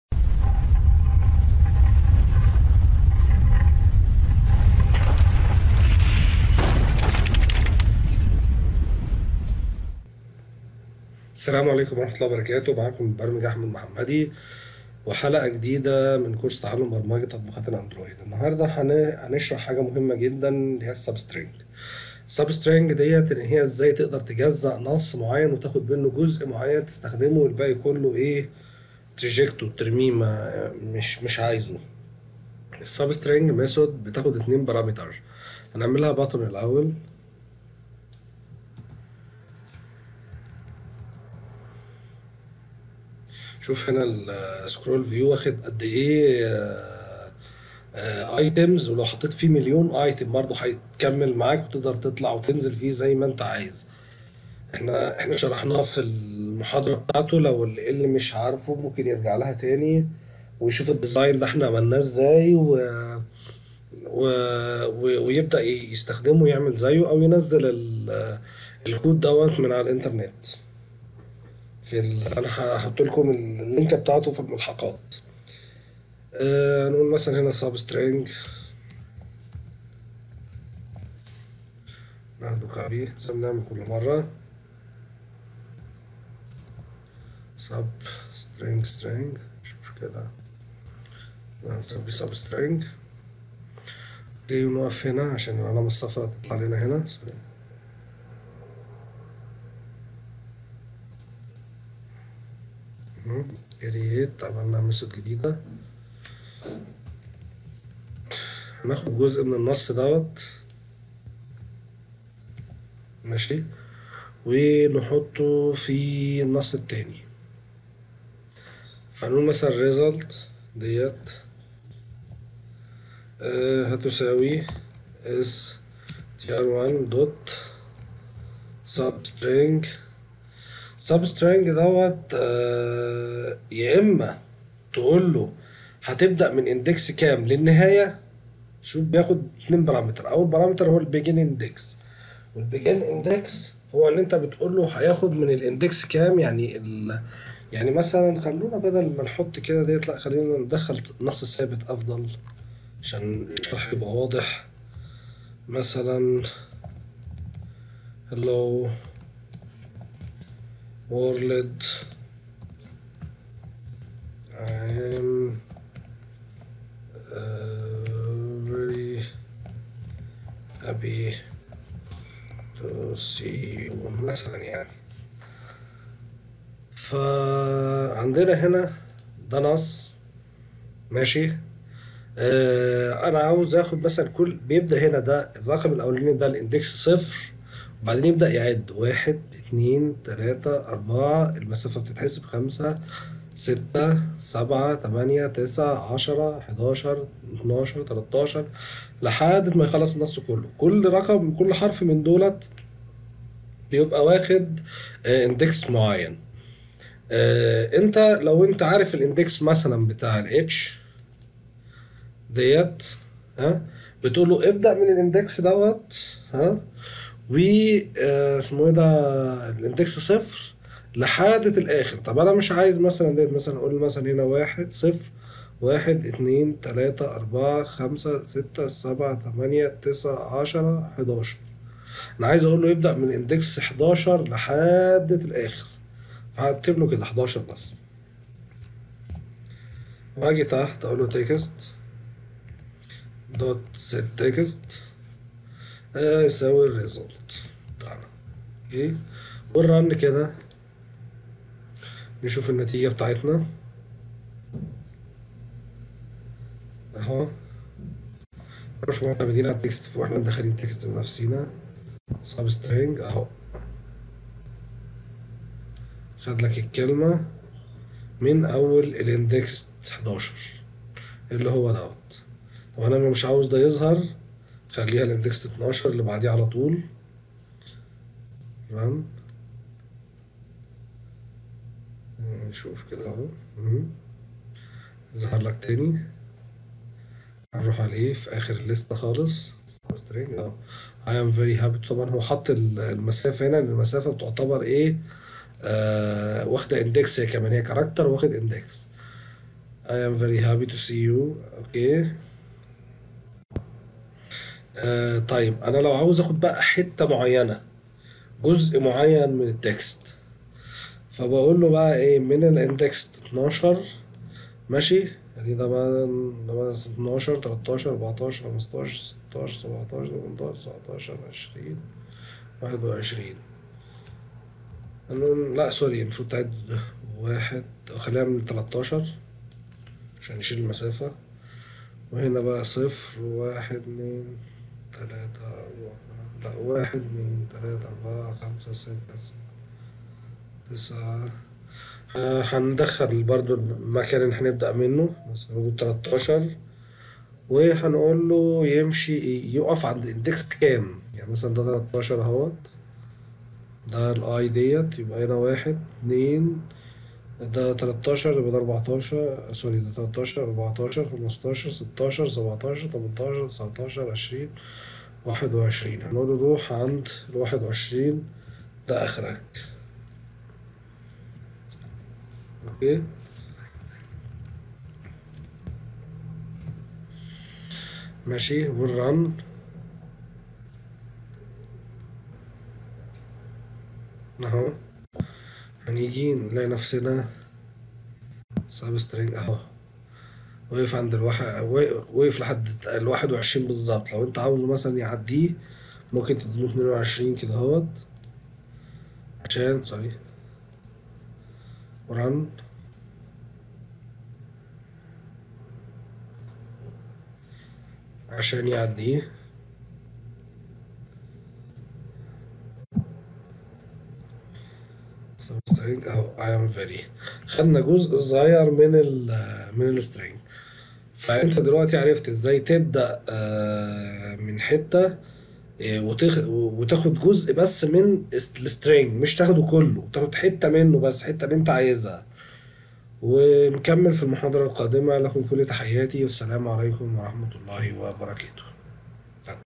30 اسرار التعامل مع المتغيرات النصية طريقة التقطيع Strings SupString
30- كورس إحتراف برمجه تطبيقات الأندرويد: المتغيرات النصية Strings التقطيع Sub String

في هذا الدرس سنقوم بشرح المتغيرات النصية Strings لتقطيع Sub String

ولا تنسي أن تعمل
للقناة
تقبلوا تحياتي
احمد محمدي